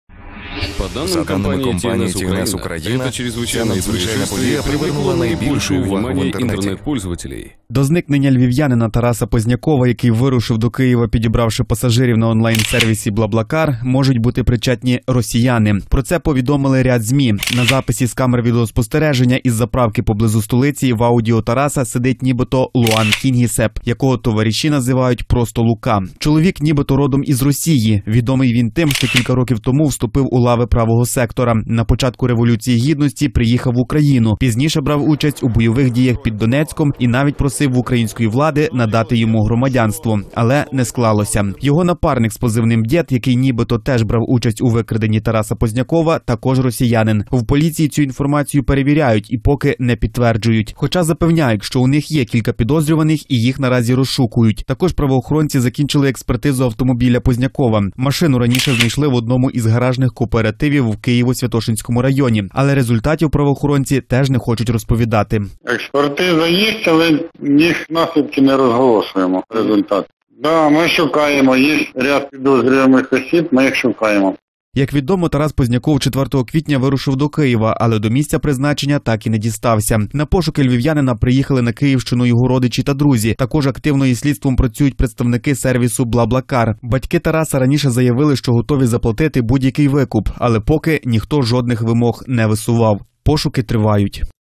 Топ Новина. До зникнення водія «BLA BLA CAR» можуть бути причетні росіяни
До зникнення львів’янина Тараса Познякова, який вирушив до Києва, підібравши пасажирів на онлайнсервісі«BLA BLA CAR», можуть бути причетні росіяни. Про це повідомили ряд ЗМІ. На записі з камер відеоспостереження із заправки поблизу столиці в машині Тараса сидить нібито  Луан Кінгісеп, якого товариші називають просто Лука. Чоловік нібито родом із Росії. Відомий він тим, що кілька років тому вступив у лави «Правого Сектора». Детальніше, дивіться у сюжеті.